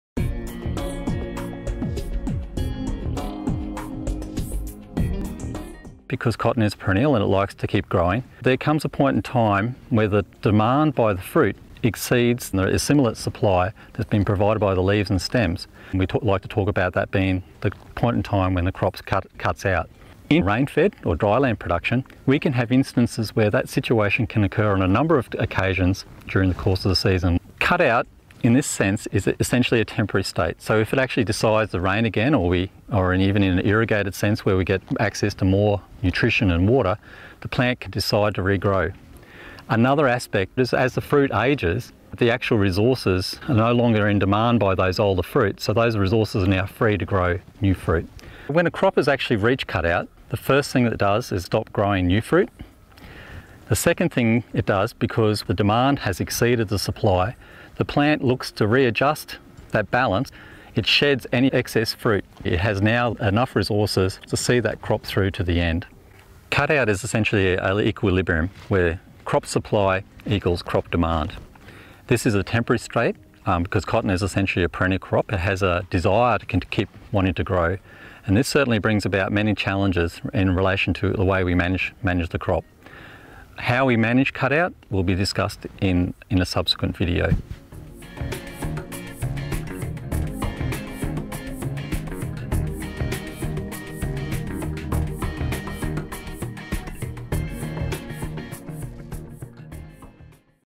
Cut-out in cotton 1: Understanding what happens
Physiologist Michael Bange (CSIRO) explains the delicate balance between resource supply and demand that causes the cotton plant to focus on filling the fruit it already has rather than setting more bolls.